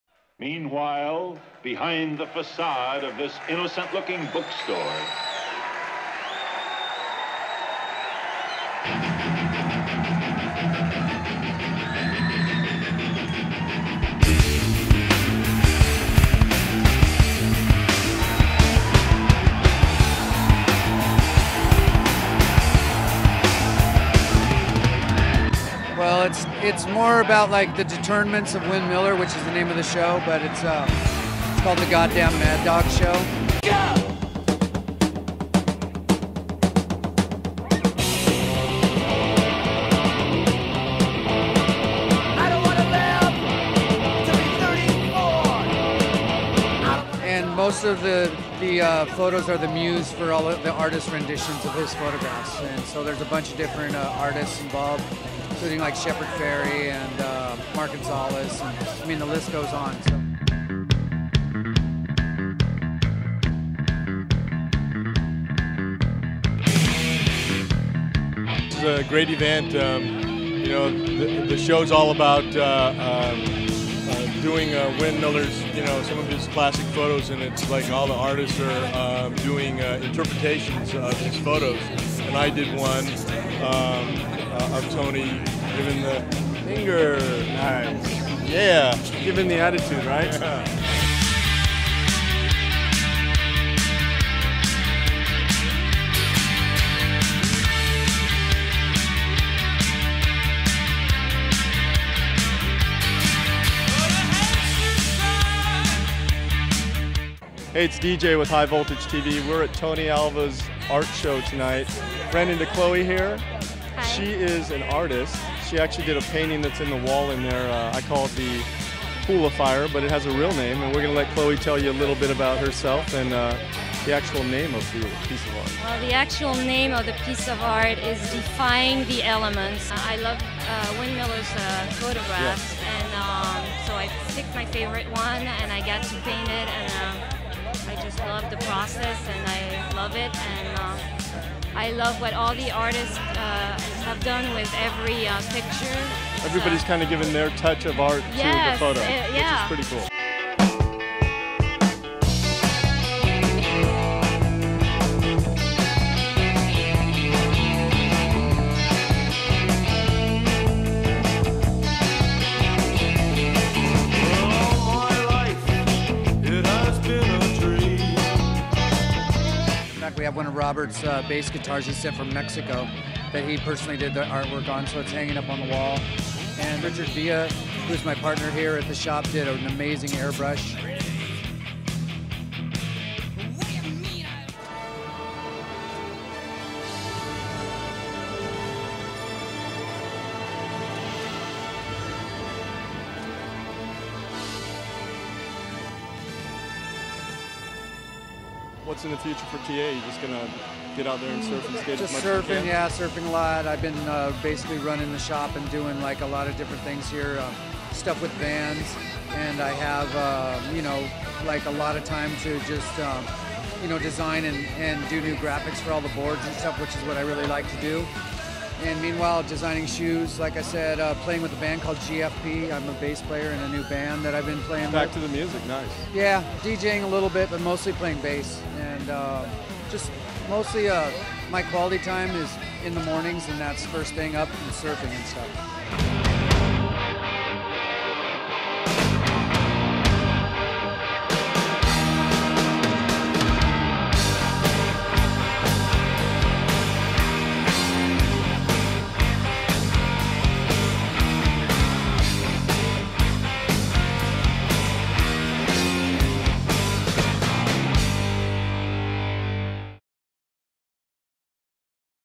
TONY ALVA /Wynn Miller MAD DOG Art Show LA 6-09 HIGH VOLTAGE TV
LA's Exhibit A Wynn Miller Art Show featuring Photography by Wynn Miller with renditions by many Top Artists.